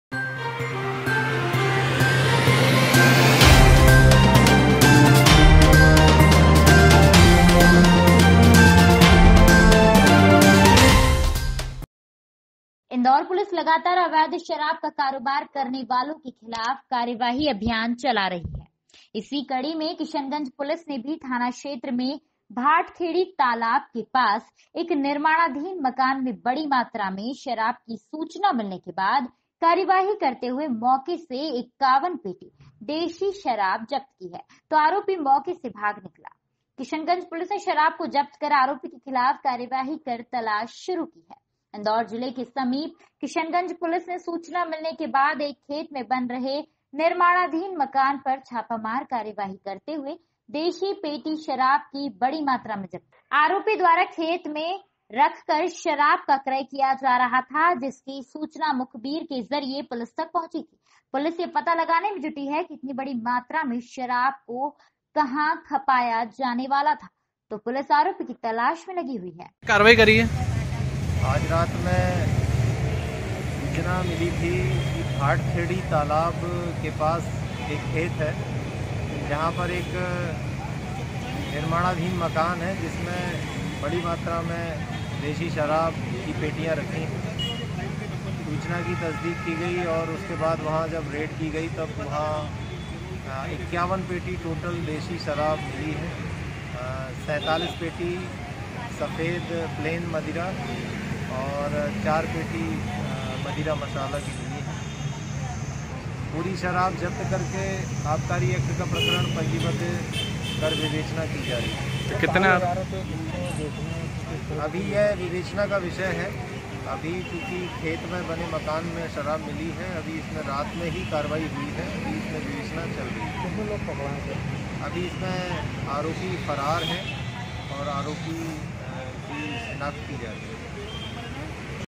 इंदौर पुलिस लगातार अवैध शराब का कारोबार करने वालो के खिलाफ कार्यवाही
इंदौर पुलिस लगातार अवैध शराब का कारोबार करने वालो के खिलाफ कार्यवाही अभियान चला रहा ही इस ही कड़ी में किशनगंज पुलिस ने भी थाना क्षेत्र में भाटखेड़ी तालाब के पास एक निर्माणधीन मकान में बड़ी मात्रा में शराब की सुचना मिलने के बाद कार्यवाही करते हुए मोके से 51 पेटी देशी शराब जब्त की हे तो आरोपी मोके से भाग निकले किशनगंज पुलिस ने शराब को जब्त कर आरोपी के खिलाफ कार्यवाही कर तलाश शुरू की हे

इंदौर जिले के समीप किशनगंज पुलिस ने सुचना मिलने के बाद एक खेत में बन रहे निर्माणधीन मकान पर छापा पार कार्यवाही करते हुए देशी पेटी शराब की बड़ी मात्रा में जब्त की हे आरोपी दवारा खेत में रखकर शराब का क्रय किया जा रहा था जिसकी सुचना मुखबिर के जरिये पुलिस तक पहुंची थी पुलिस यह पता लगाने में जुटी हे की इतनी बड़ी मात्रा में शराब को कह खपाया जाने वाला था  तो पुलिस आरोपी की तलाश में लगी हे